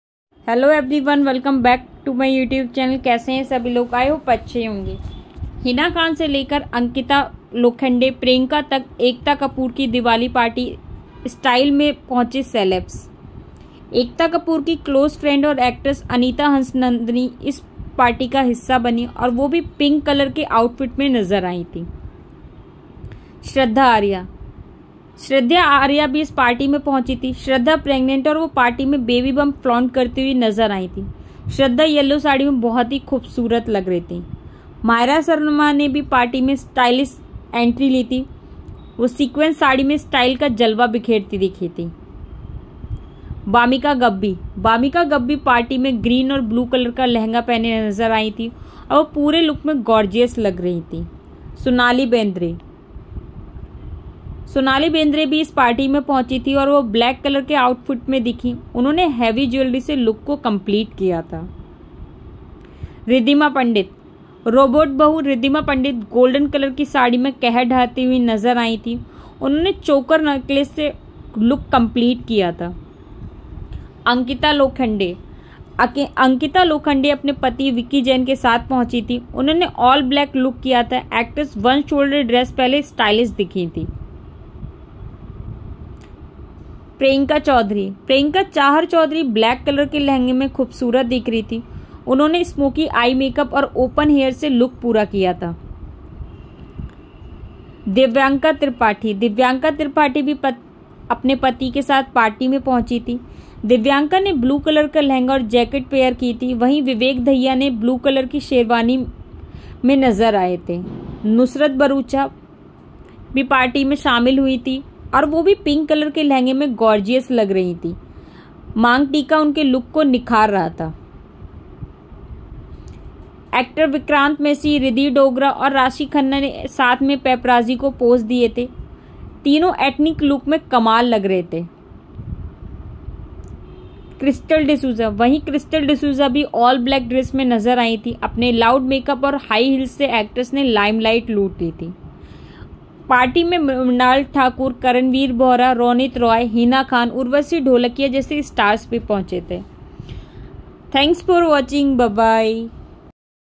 हिना खान से लेकर अंकिता- प्रियंका तक, एकता कपूर की दीवाली पार्टी में स्टाइल में पहुंचे सेलेब्स
हिना खान से लेकर अंकिता- प्रियंका तक, एकता कपूर की दीवाली पार्टी में स्टाइल में पहुंचे सेलेब्स, देखें Pics